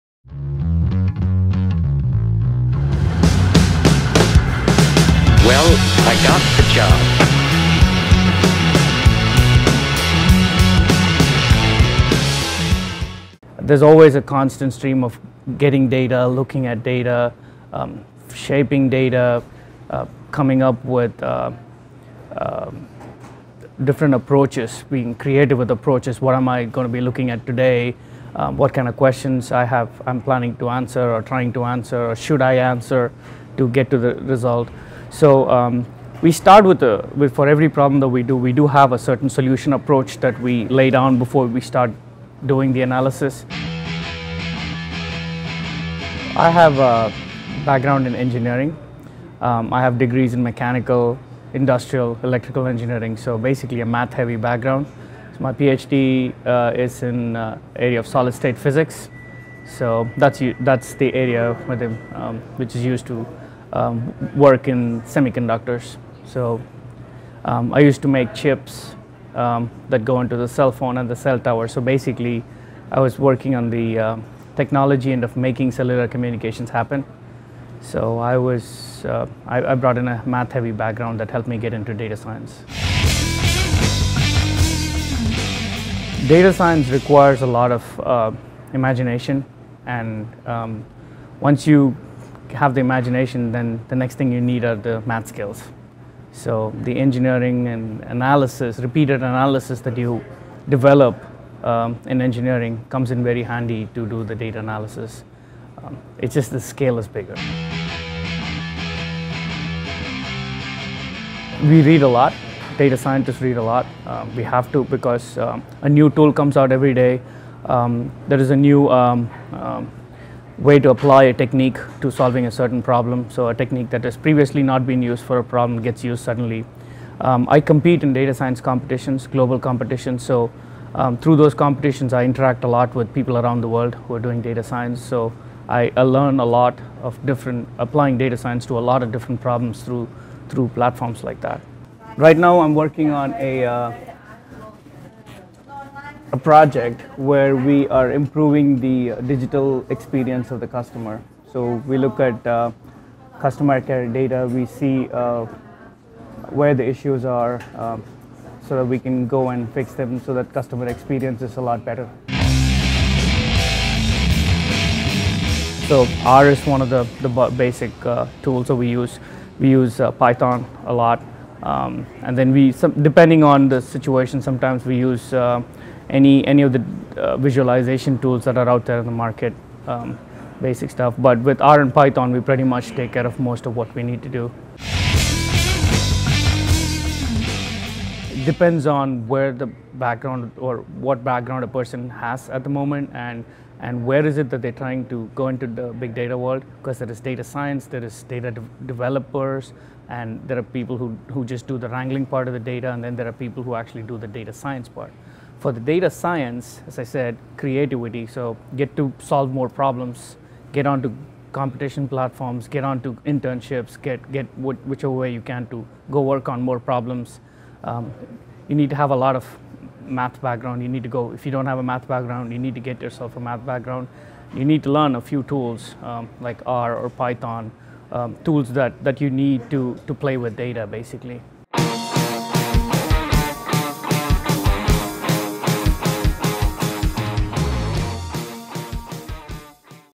Gigs: A day in the life of a data scientist
In this edition of "Gigs," RCRtv takes a look at a day in the life of a data scientist at the AT&T Foundry in Plano, Texas.